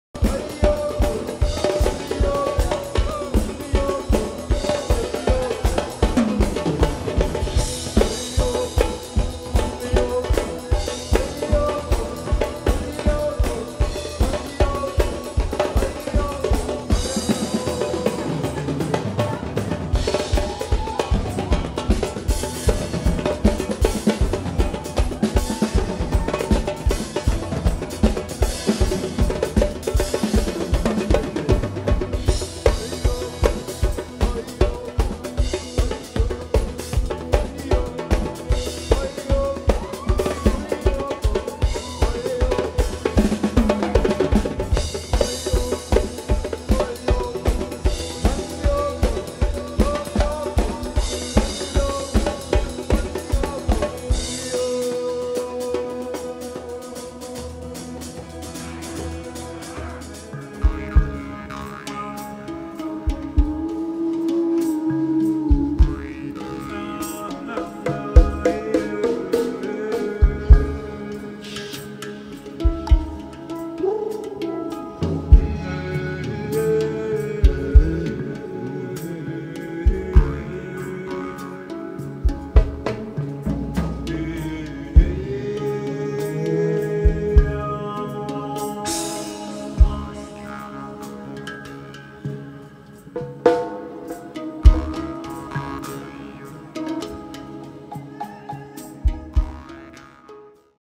2021-10 Trance Dance in Biel, CH. Rishi & Shakya
This is a clip from last autumn in Biel, Switzerland. We first started with songs and a Cacao Ceremony and then tranfered into a TranceDance. A beautiful tribe of dancers. Thank you to Jerome for making it happen...